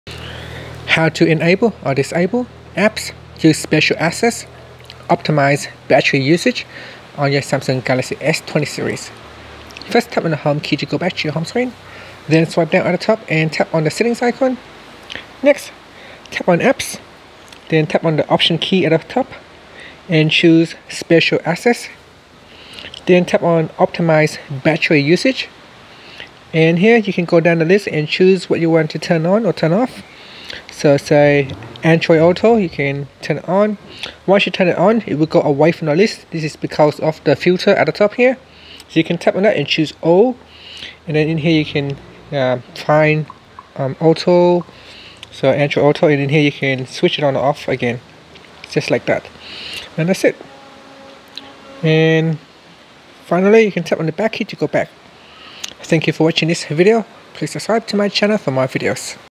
Galaxy S20/S20+: How to Enable/Disable Apps to Special Access Optimize Battery Usage
Learn how you can enable or disable apps to Special Access Optimize Battery Usage on Galaxy S20 / S20 Plus / S20 Ultra.

Android 10.